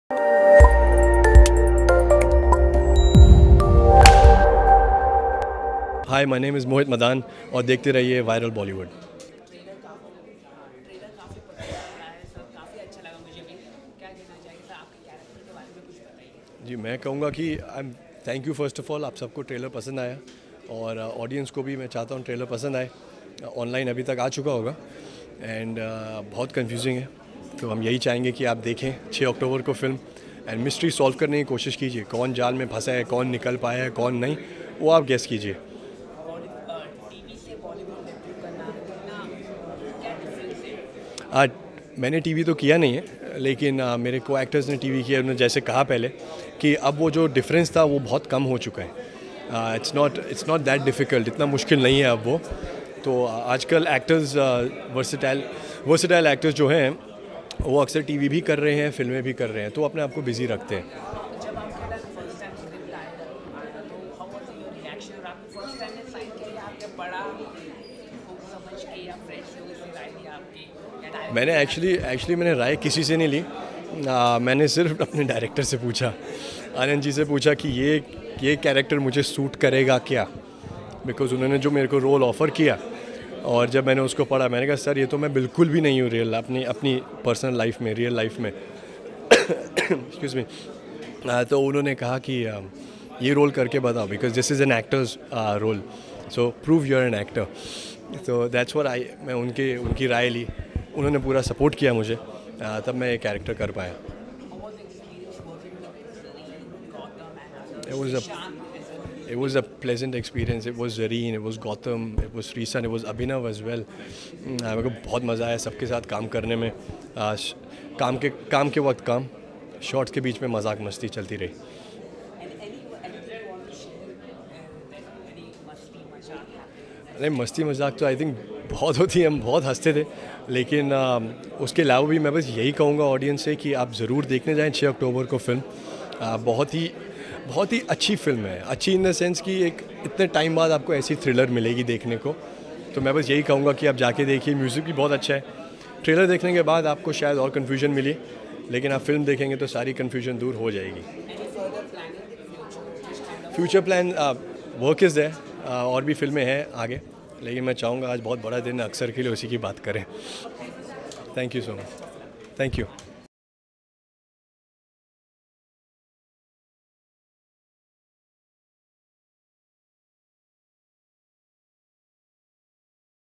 Mohit Madaan Exclusive Interview | Aksar 2 Official Trailer Launch
Watch Mohit Madaan Exclusive Interview | Aksar 2 Official Trailer Launch